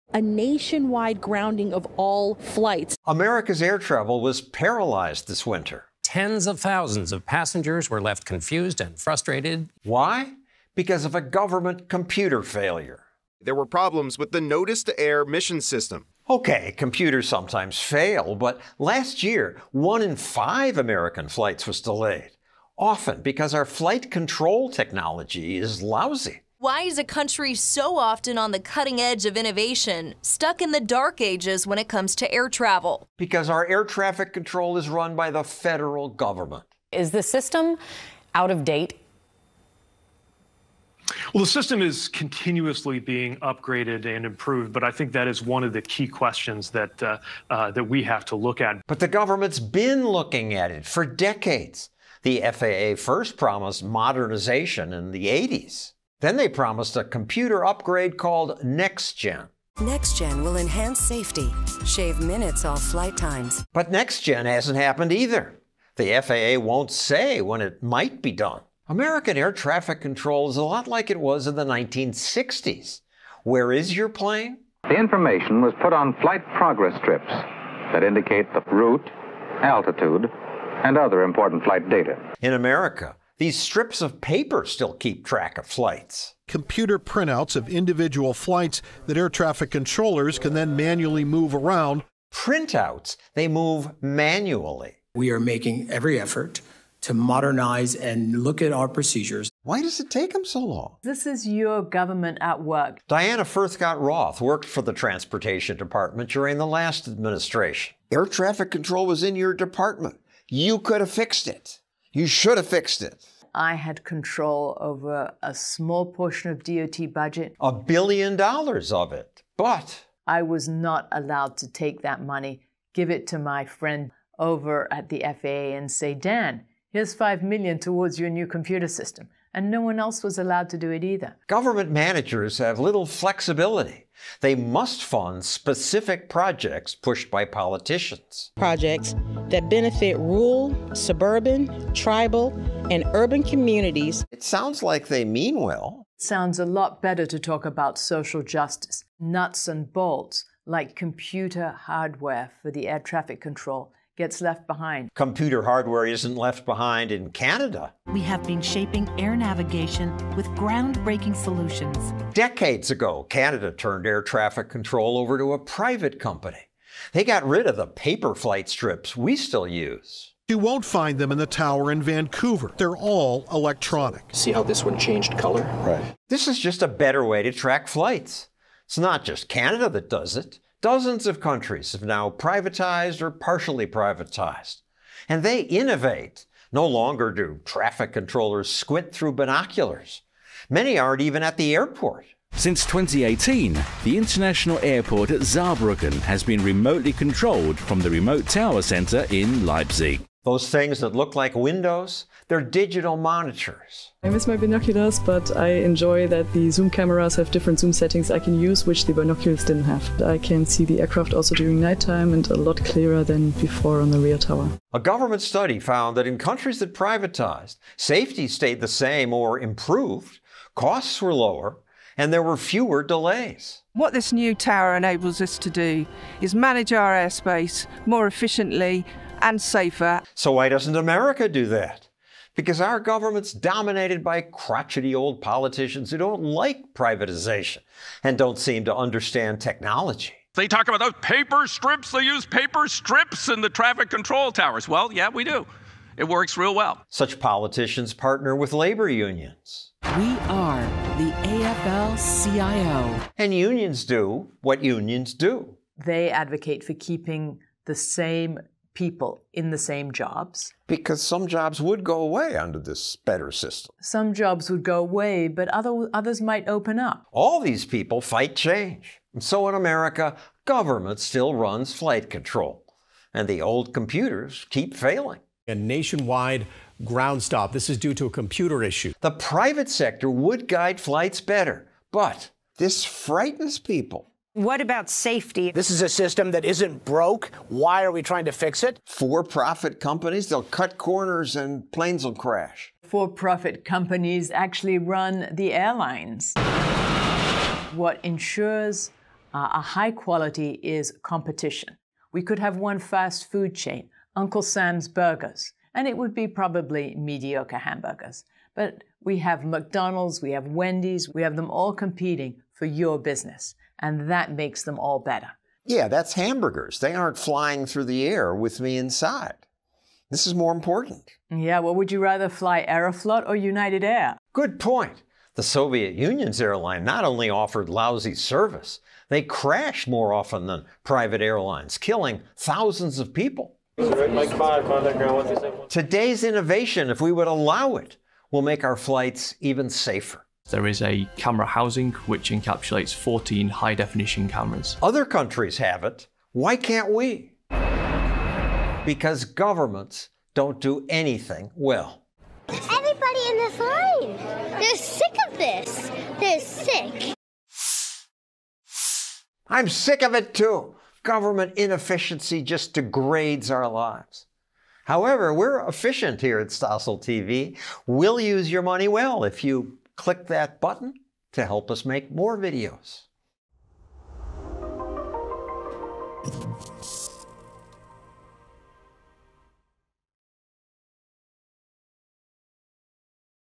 Privatize Air Traffic Control!
Private air traffic control would be faster, cheaper and maybe safer.

To get our new weekly video from Stossel TV, sign up

In America, air traffic control is run by the federal government.

Since government doesn’t do anything well, our flight control technology is lousy.

Earlier this year, a government computer failure grounded flights nationwide.

For decades, the FAA has promised to modernize the system.

But it never happens.   Government at work.

Private air traffic control works well in other countries.

Why can’t we have it here?